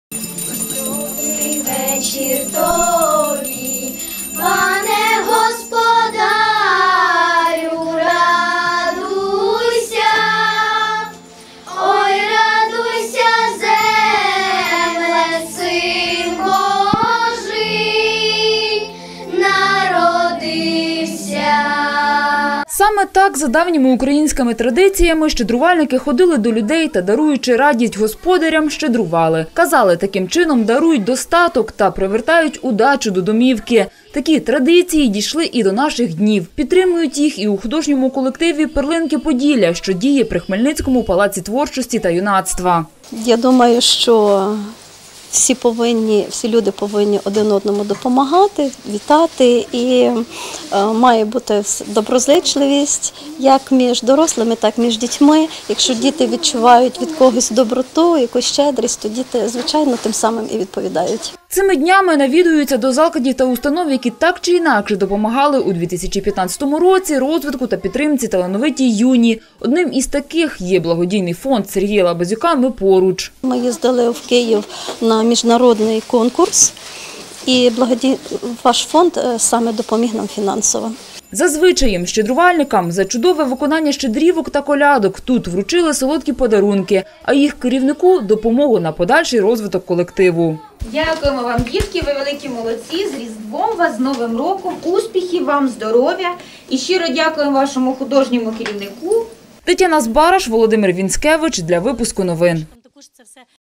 Щедрування у Хмельницькому
18 січня країна відзначила другий Святвечір, або як його ще називають «Голодна кутя». Відзначається напередодні Водохреща, за пісними стравами  мовлять молитву та з першою зіркою куштують кутю. А ще діти щедрують.